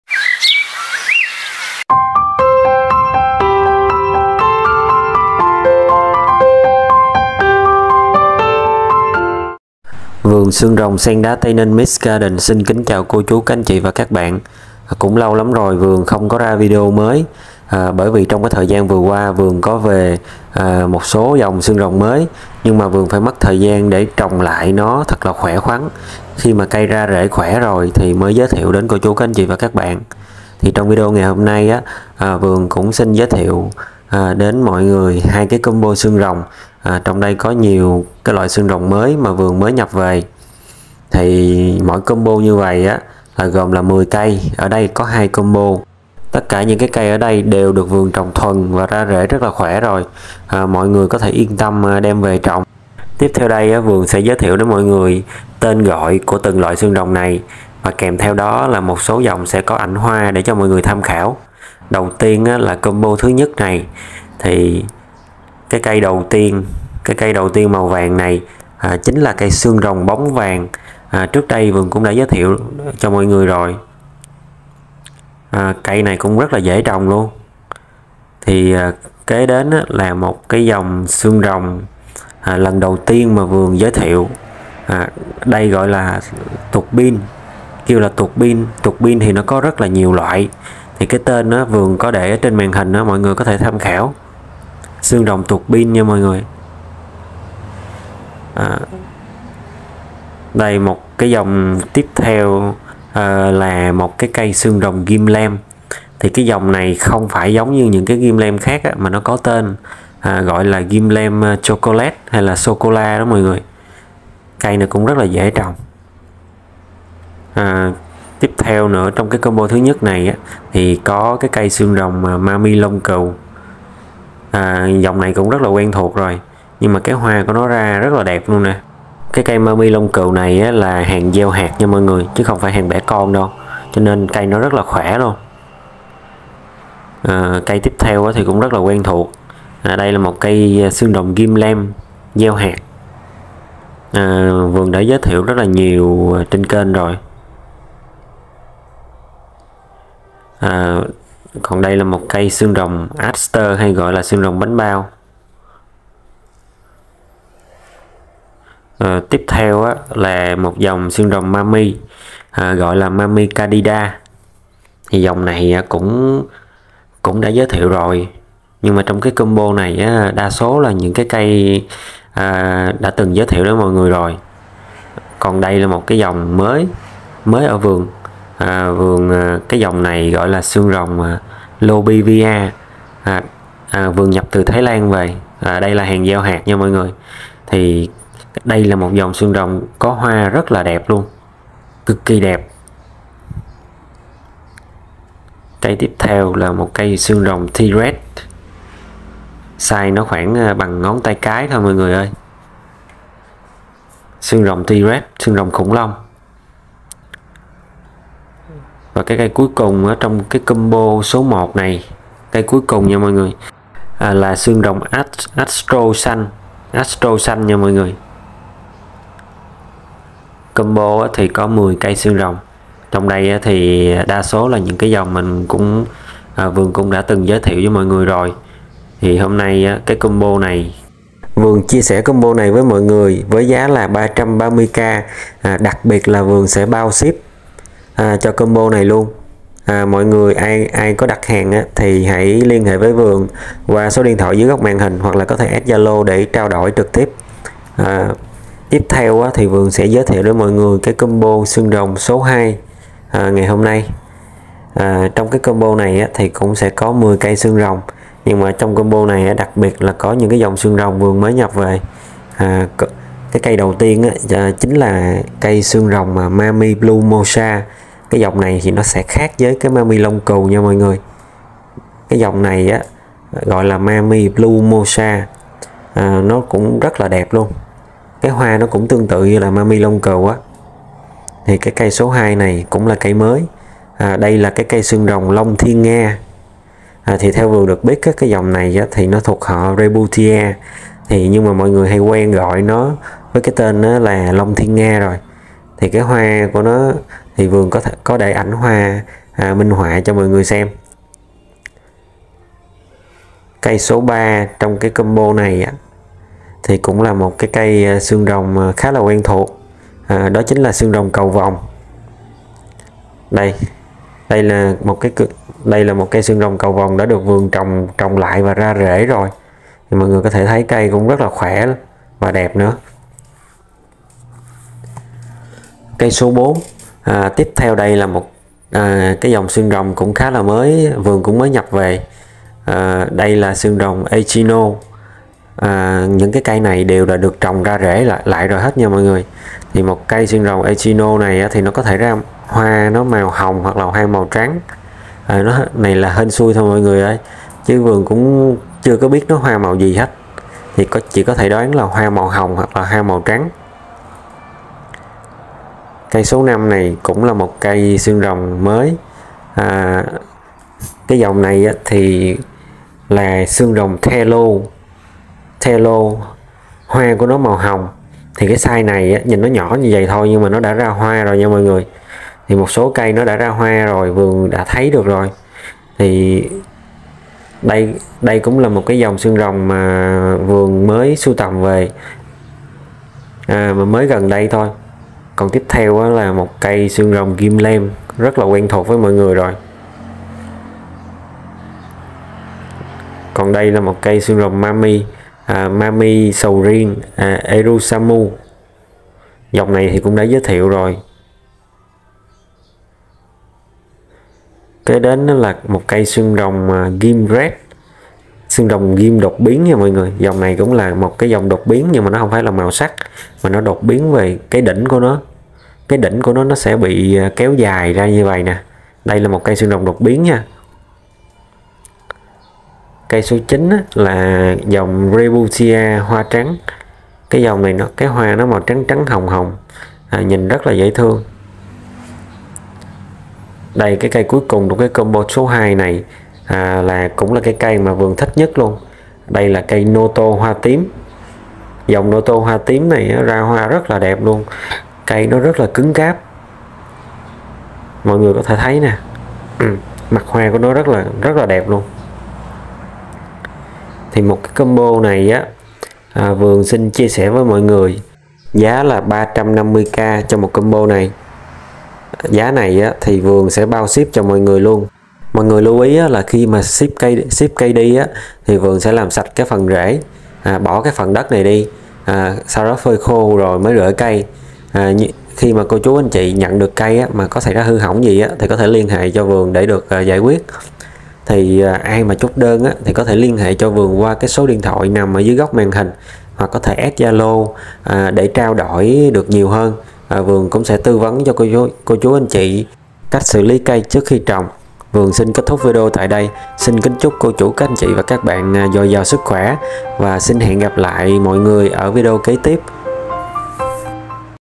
Xương Rồng Giống Mới Đã Trồng Thuần Khí Hậu Tây Ninh
COMBO XƯƠNG RỒNG GIỐNG MỚI

Đây là các giống xương rồng giống mới được trồng thuần tại khí hậu Tây Ninh. Mỗi combo gồm 10 cây dành cho người mới chơi xương rồng kèm theo sự hỗ trợ tư vấn chăm sóc trong suốt quá trình trồng.